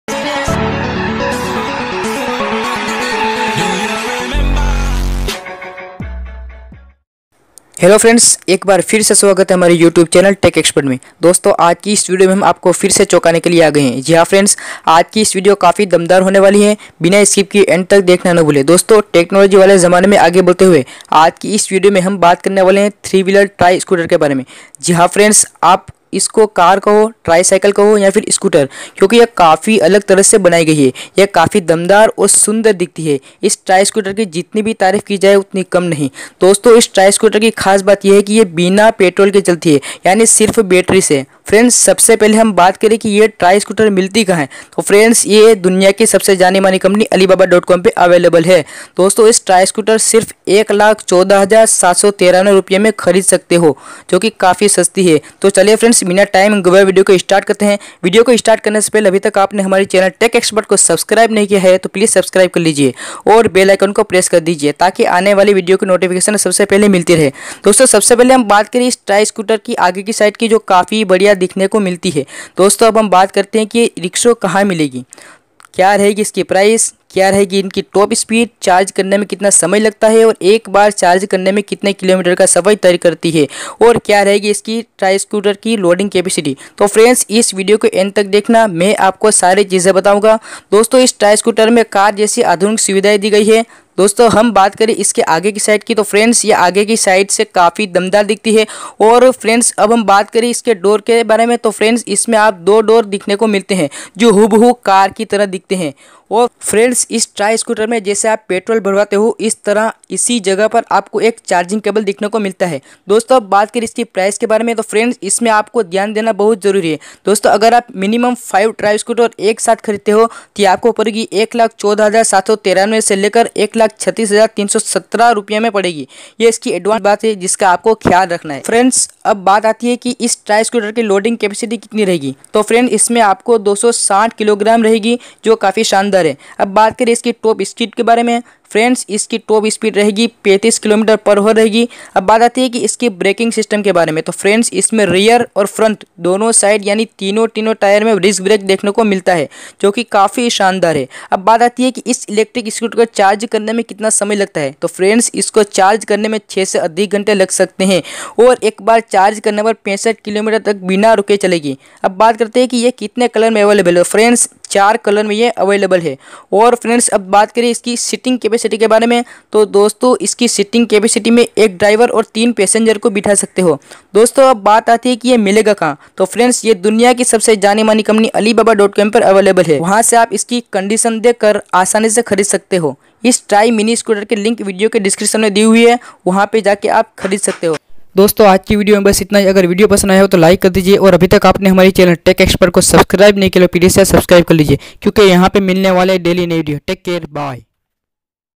लॉन्च होते ही लोगो के दिलो में छा गई ये ट्राई मिनी बाइक किंमत जानकर होश उड़ जाएंगे।
three wheel cargo tricycle electric tricycle with passenger seats battery rickshaw

यहां मिलती है ये bike {click now to Alibaba.com}

Website Link for Universeecar